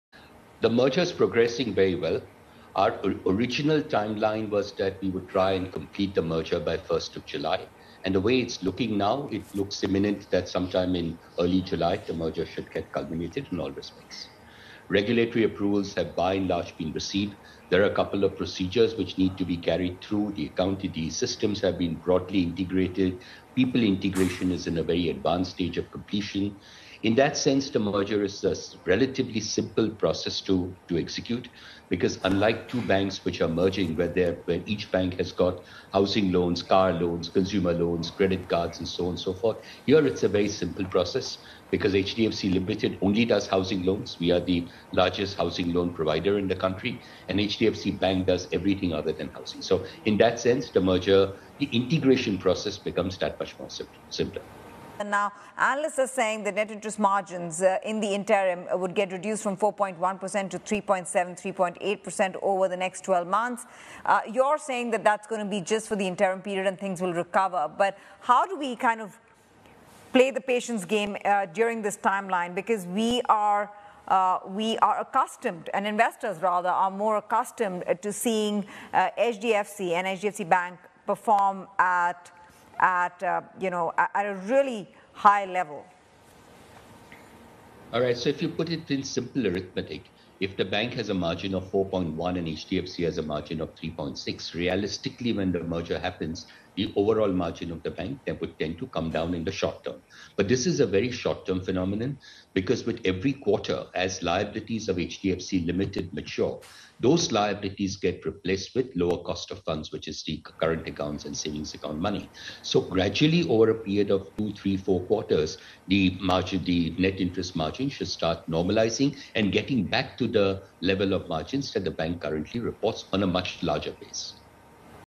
HDFC-HDFC Bank merger: It's progressing very well, says HDFC CEO
Keki Mistry of the housing finance company says the merger should be completed by early July.